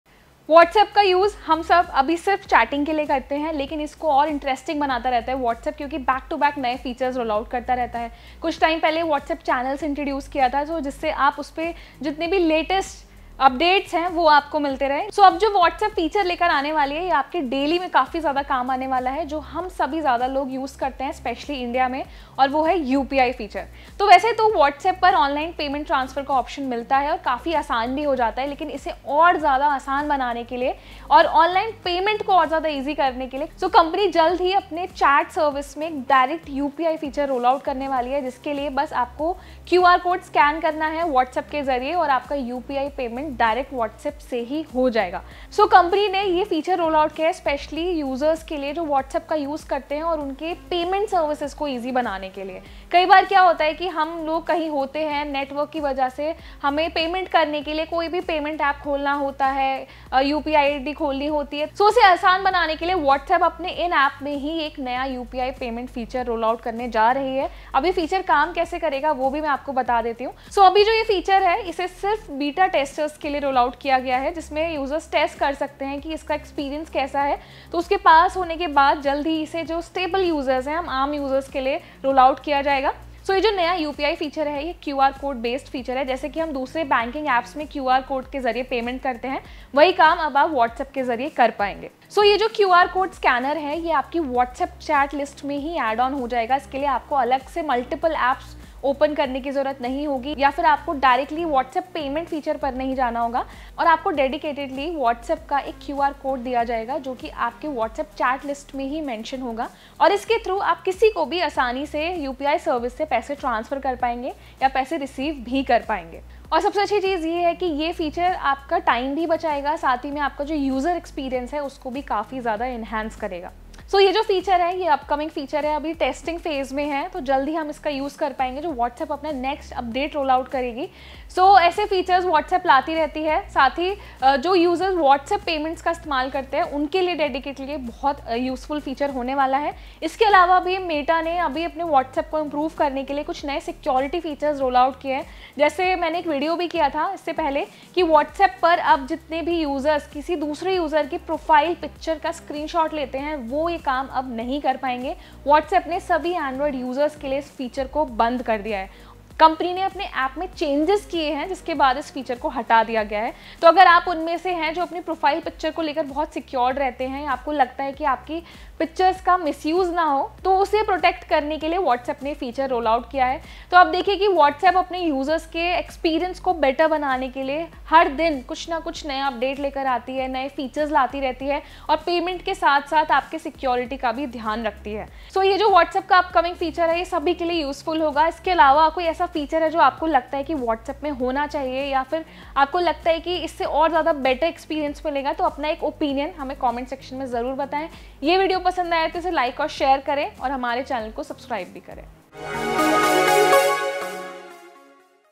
WhatsApp से एक क्लिक में हो जाएगी UPI पेमेंट, आ रहा है नया Feature | NBT Tech-ED
WhatsApp introduces a new payment feature enabling users to scan UPI QR codes directly from their chat list, accelerating payment processes. Initially spotted by WAbetaInfo, the feature is accessible to Android beta testers, streamlining transactions with a convenient shortcut in the chats list.

व्हाट्सएप ने एक नया पेमेंट फीचर पेश किया है जो यूजर्स को अपनी चैट लिस्ट से सीधे यूपीआई क्यूआर कोड को स्कैन करने में सक्षम बनाता है| इससे यूजर्स को पेमेंट करने में आसानी होगी।